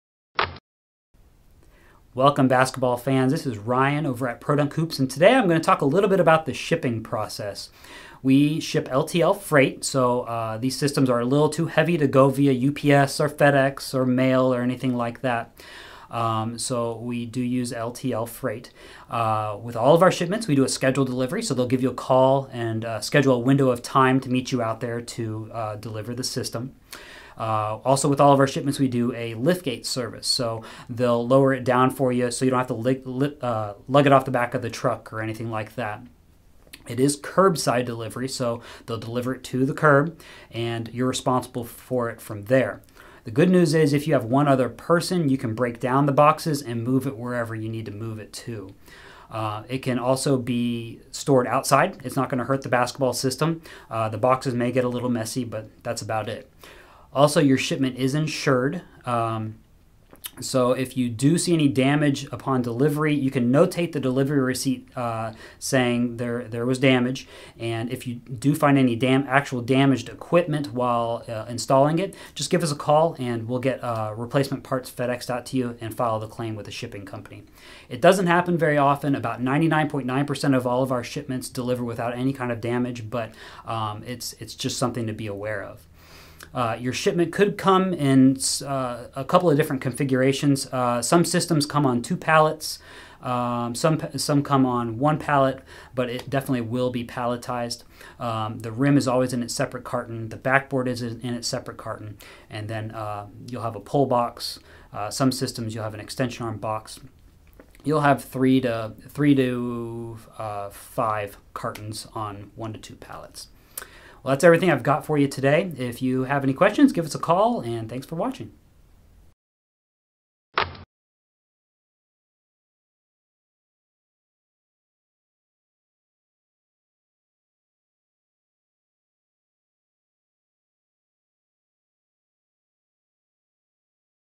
Pro Dunk® Basketball Goal Shipping Overview
We get a lot of questions about the shipping process so we put together this video to address questions such as, "when should I expect my delivery?"  We'll go over details such as tracking numbers, what kind of shipping company we use, lift-gate service, curb-side delivery and how to check for shipping damage.  Anything you want to know about shipping should be right here in this video.  It's a must watch for anybody expecting a Pro Dunk® basketball goal soon.